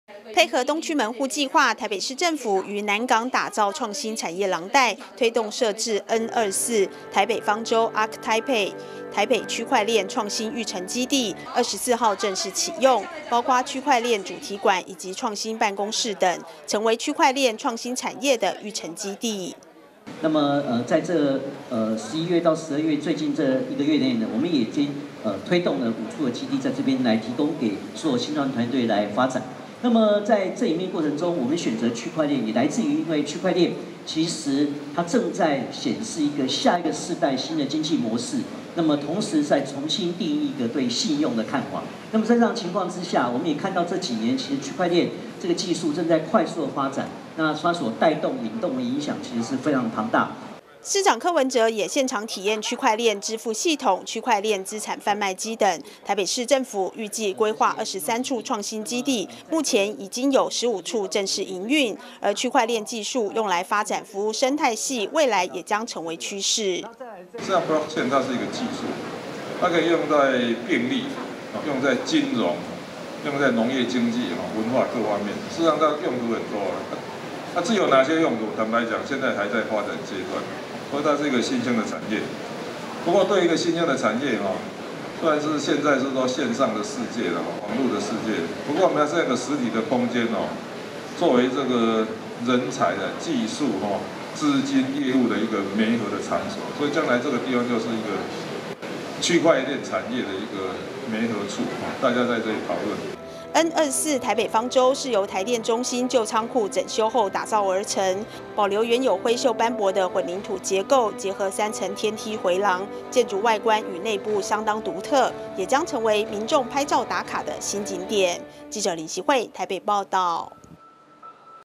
區塊鏈|柯文哲|台北方舟ark tpe|比特幣|「N24台北方舟」 區塊鏈育成基地啟用
區塊鏈|柯文哲|台北方舟ark tpe|「N24台北方舟」 區塊鏈育成基地啟用
台北市網紅新景點！台北區塊鏈創新育成基地「N24台北方舟」正式啟用

(記者林錫惠/台北報導)
台北市又多了一處網紅景點！位在南港區的台北區塊鏈創新育成基地「N24台北方舟」24號正式啟用，是由台電中心舊倉庫整修後打造而成，供新創團隊交流、辦公，由於建築外觀與內部相當獨特，成為拍照打卡新景點。

晚間6:30敬請鎖定有線電視第4頻道~

晚間6:00敬請鎖定有線電視第4頻道~